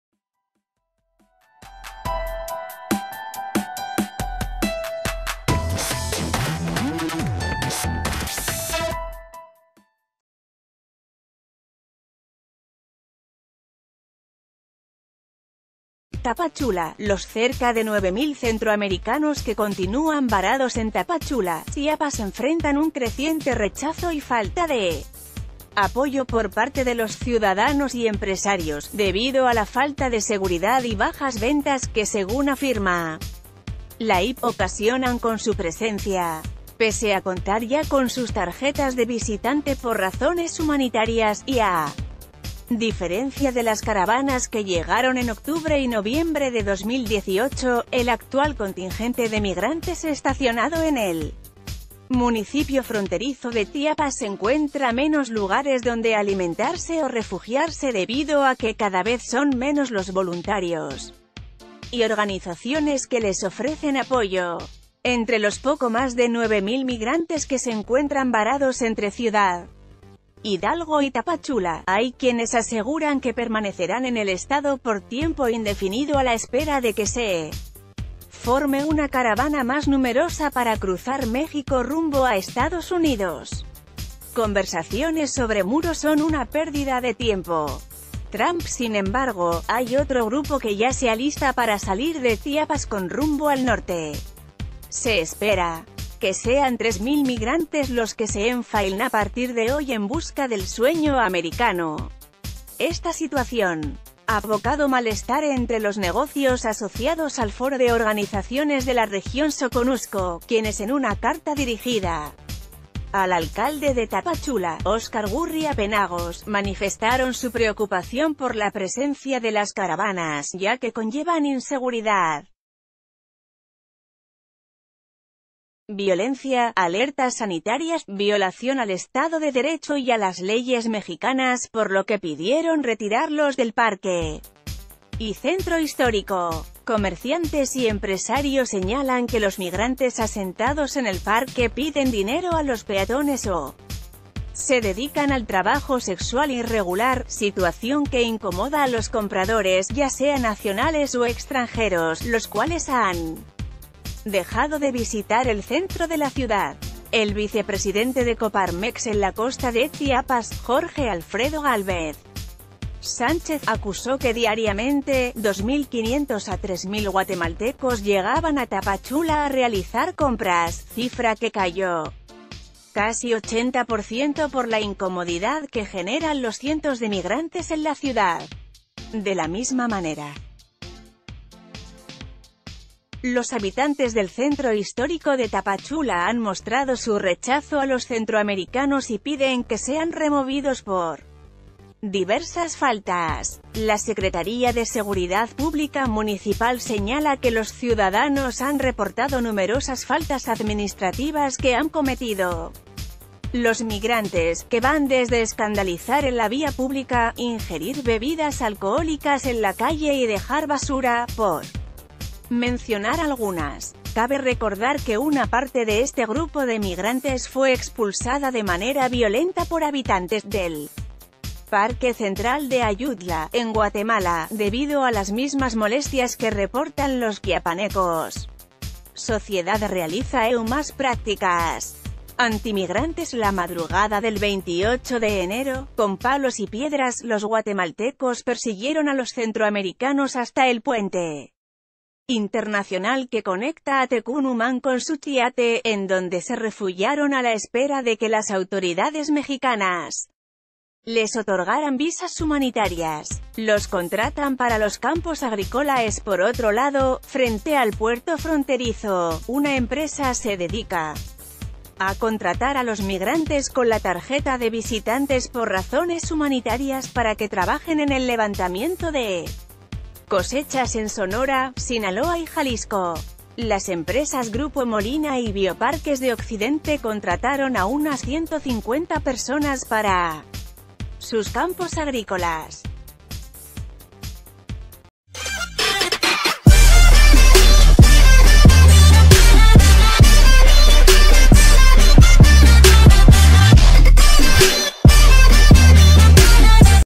Rechazo unánime de IP y ciudadanía a nueva caravana migrante
TAPACHULA.- Los cerca de nueve mil centroamericanos que continúan varados en Tapachula, Chiapas enfrentan un creciente rechazo y falta de apoyo por parte de los ciudadanos y empresarios, debido a la falta de seguridad y bajas ventas, que según afirma la IP, ocasionan con su presencia. Pese a contar ...